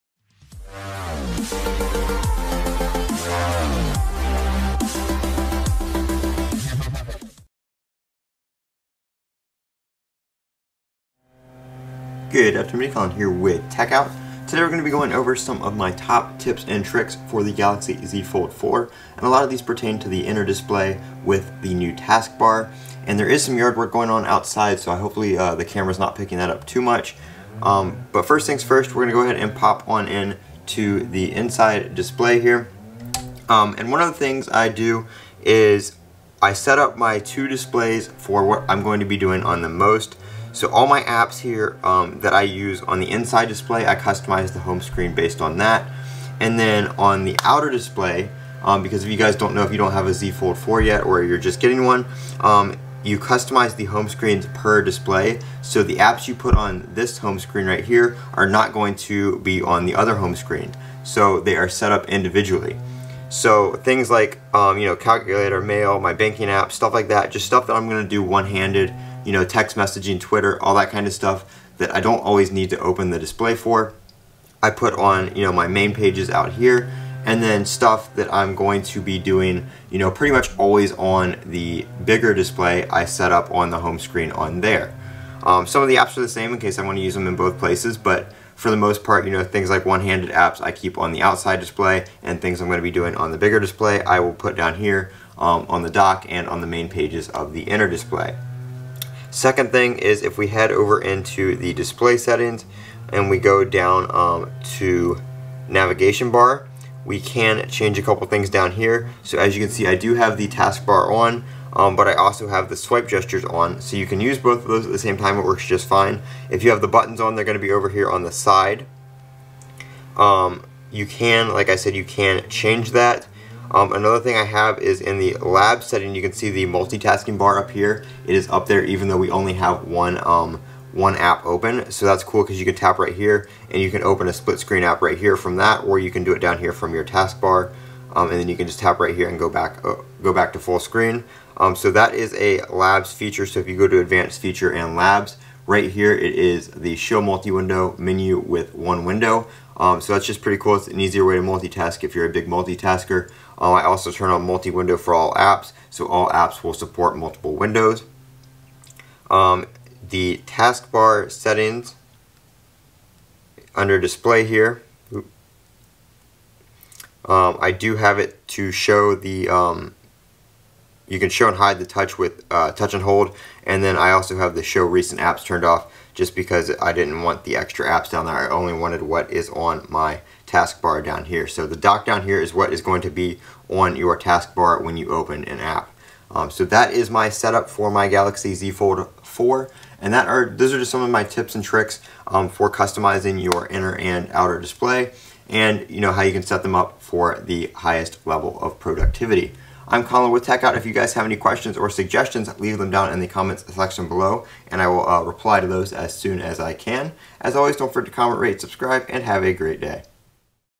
Top Galaxy Z Fold 4 Tips and Tricks For Multitasking and Home Screen Setup
14 Inch M1 MacBook Pro